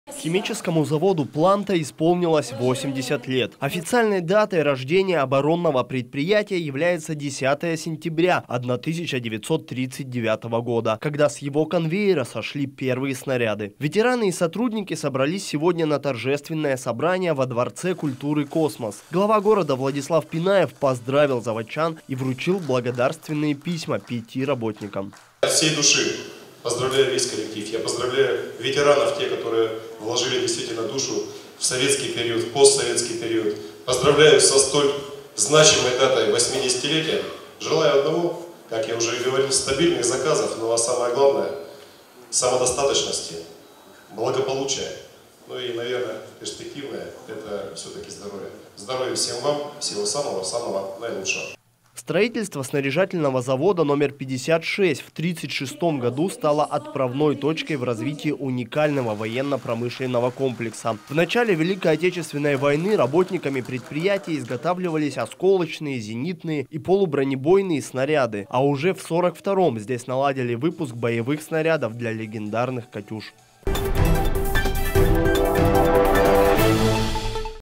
80 лет исполнилось химическому заводу "Планта"
Химическому заводу "Планта" исполнилось 80 лет. Официальной датой рождения оборонного предприятия является 10 сентября 1939 года, когда с его конвейера сошли первые снаряды. Ветераны и сотрудники собрались на торжественное собрание во дворце культуры "Космос". Глава города Владислав Пинаев поздравил заводчан и вручил благодарственные письма пяти работникам.